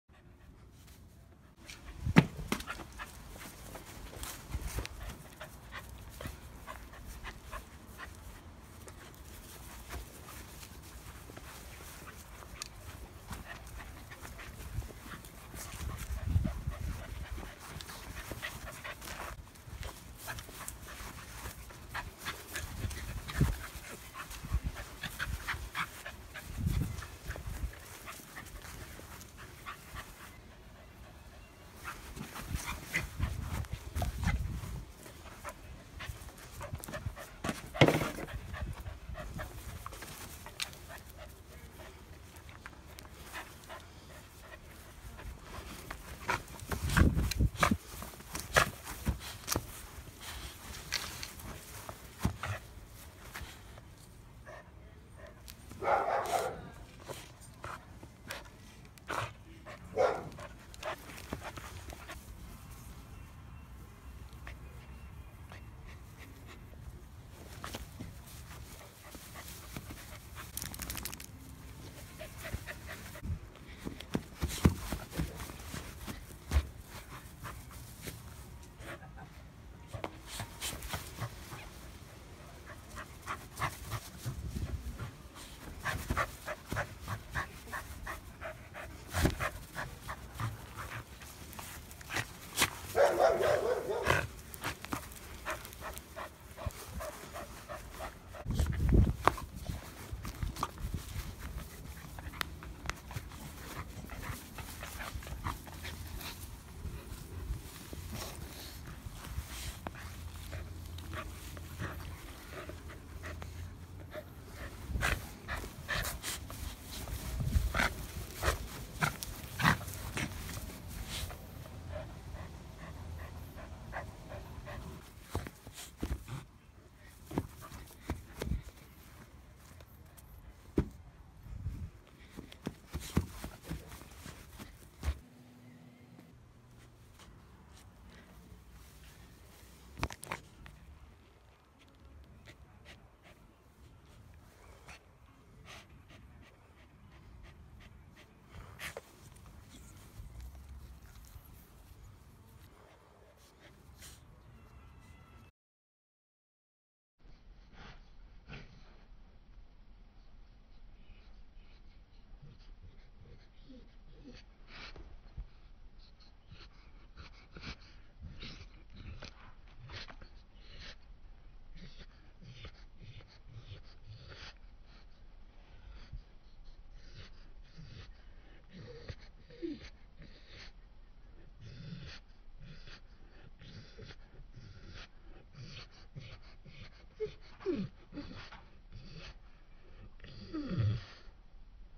Alien vs. Corgi
"Alien vs. Corgi" in honor of the release of Ridley Scott's Alien: Covenant. Epic battle with Alien face hugger. Imagine the horror of a corgi Xenomorph? Ends with a teaser to Alien vs. Pug.